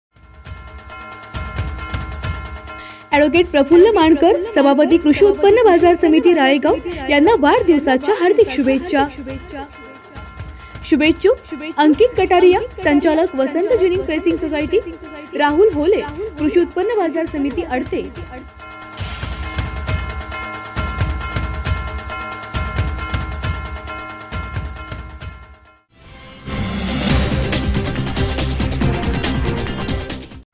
ADV आदरणीय ॲड. प्रफुल्लभाऊ मानकर यांना वाढदिवसाच्या हार्दिक शुभेच्छा...! SBN MARATHI
यवतमाळ जिल्ह्यातील सहकार नेते राळेगाव कृषी उत्पन्न बाजार समितीचे सभापती आदरणीय ॲड. प्रफुल्लभाऊ मानकर यांना वाढदिवसाच्या हार्दिक शुभेच्छा...!
शुभेच्छुक
अंकित कटारिया : संचालक वसंत जिनींग प्रेसींग सोसायटी,  राहुल होले : कृषी उत्पन्न बाजार समिती अडते